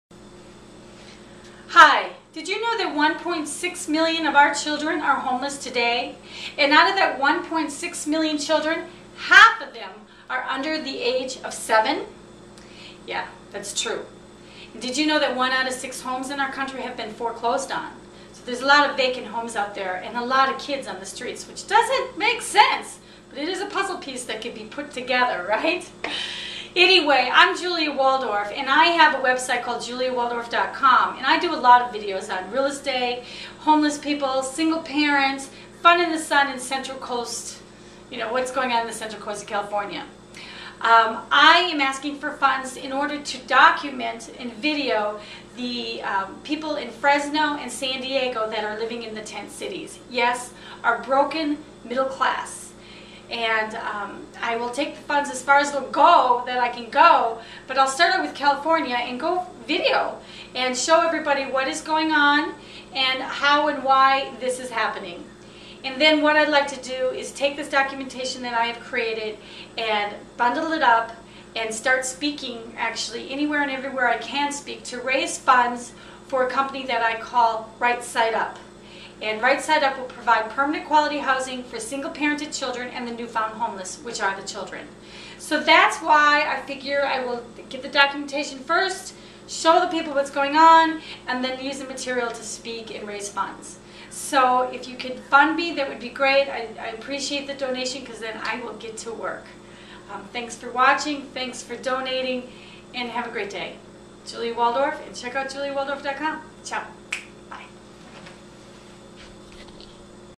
Right Side Up!
putting together a fund raiser campaign for indiego in order to raise funds to do documentation of the broken middle class.